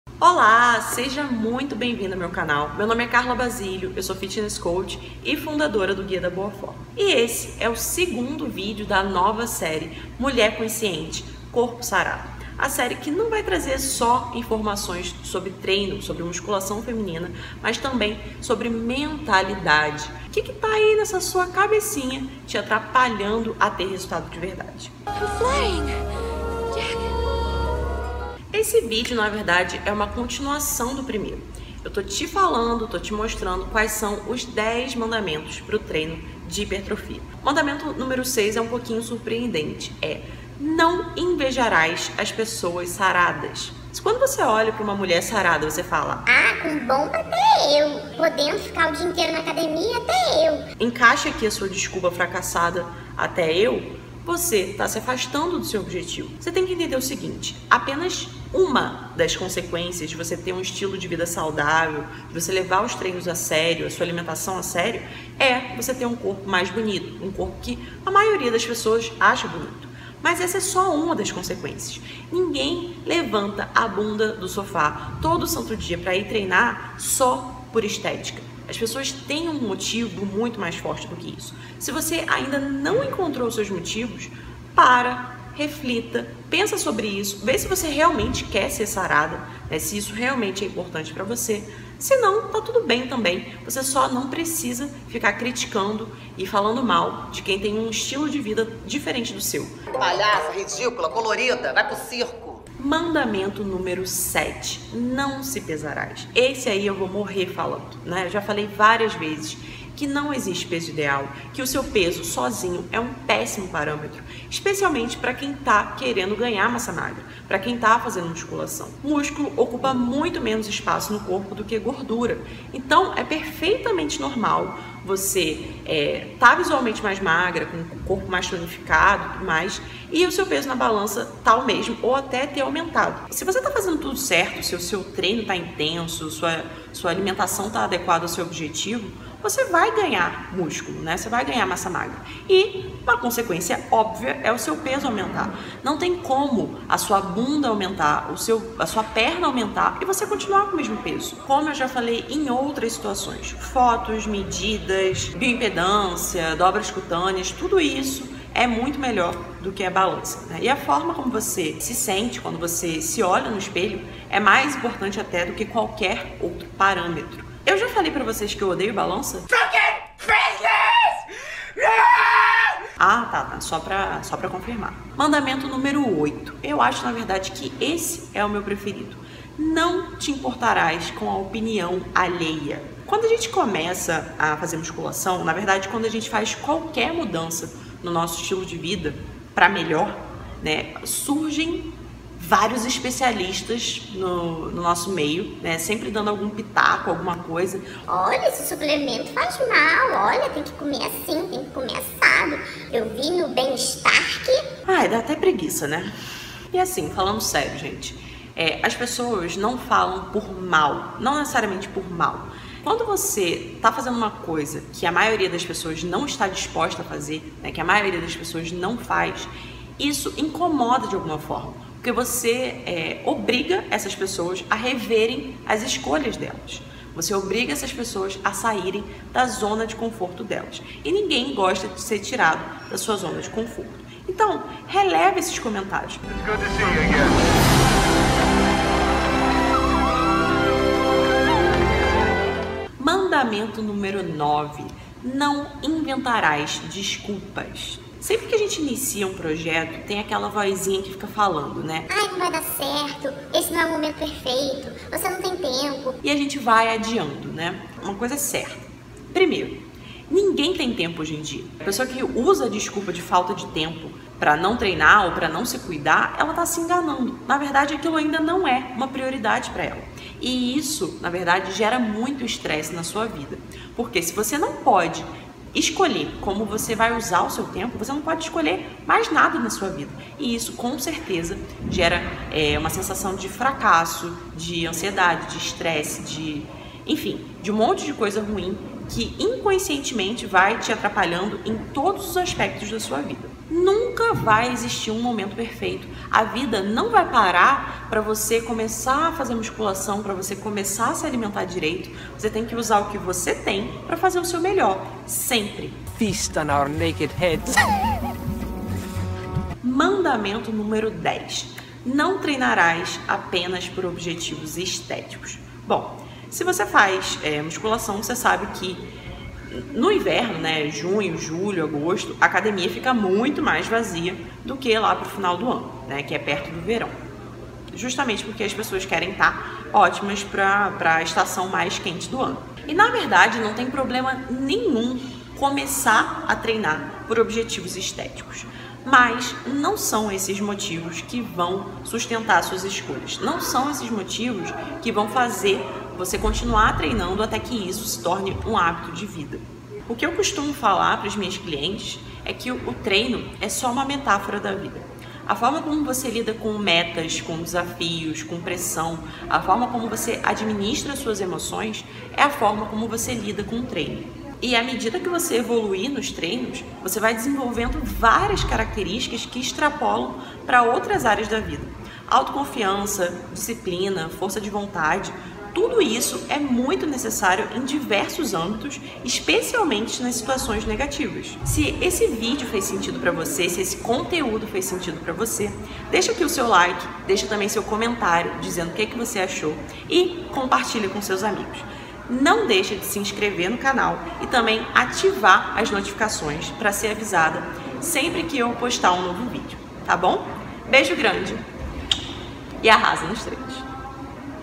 TREINO PARA HIPERTROFIA: Os 10 Mandamentos da Mulher Sarada - PARTE 02 | CARLA BASÍLIO
Dando sequência a nova série do canal, MULHER CONSCIENTE CORPO SARADO 💪. Essa é a segunda parte dos 10 mandamentos da mulher sarada. Assista agora!

Ebook (60 Opções de Pré e Pós-Treino)

Produção:
Gostart Digital